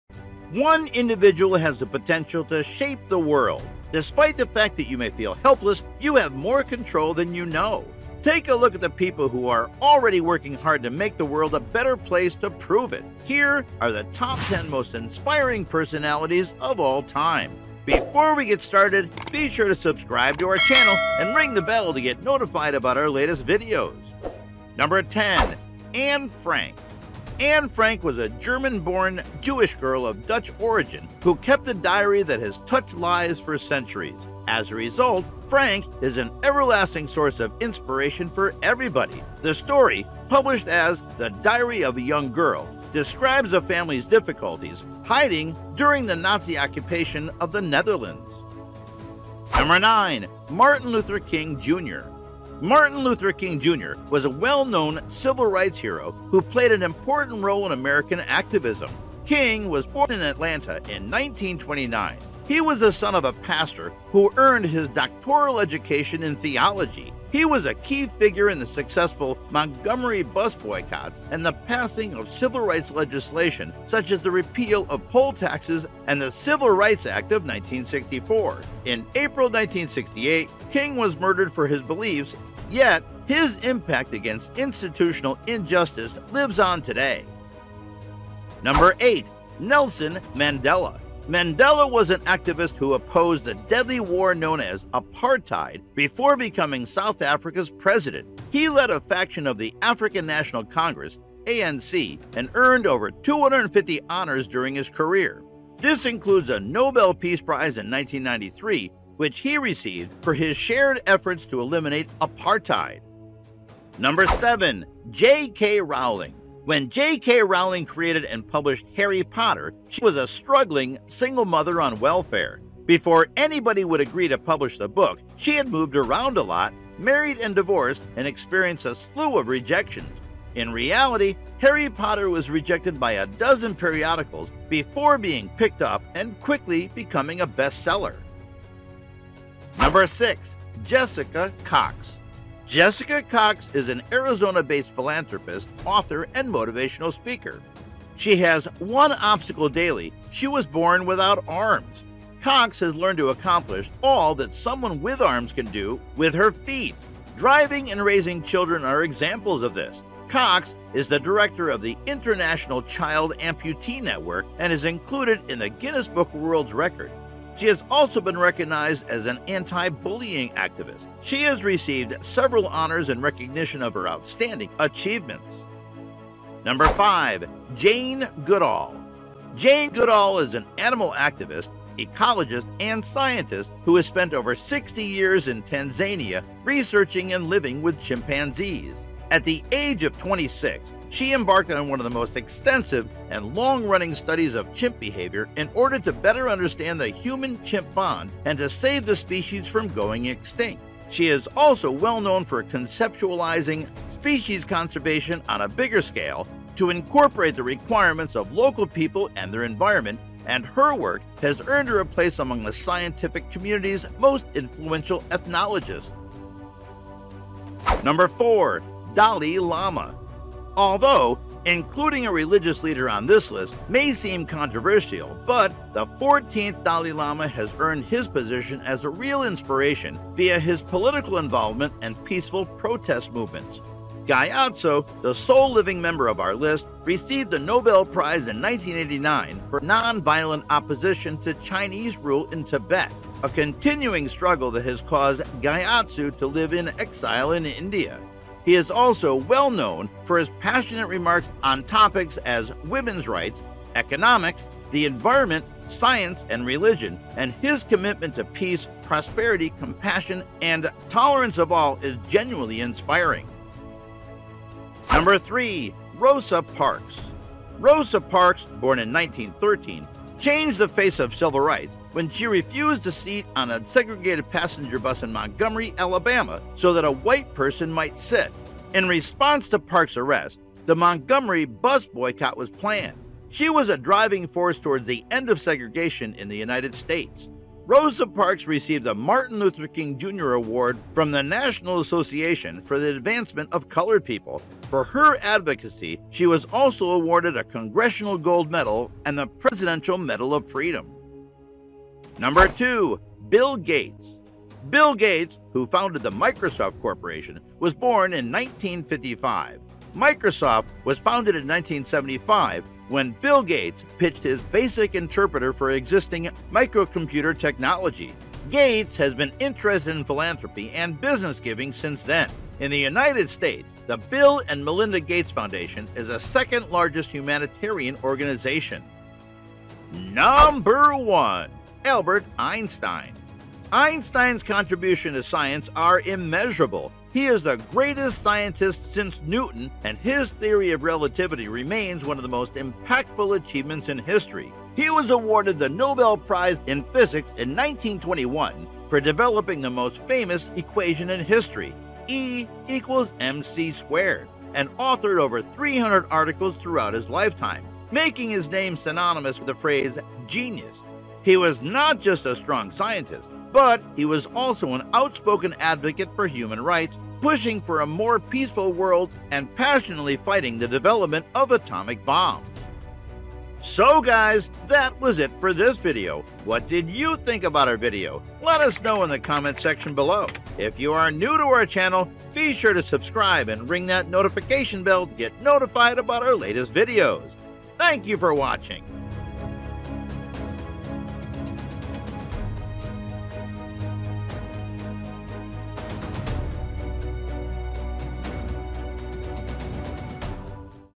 Top 10 most inspiring personalities of all time
Hey Guys Welcome to “Top 10 Rankings”. Despite the fact that you may feel helpless, you have more control than you know. Take a look at the people who are already working hard to make the world a better place to prove it. Here are Top 10 most inspiring personalities of all time. So let’s take a view….
e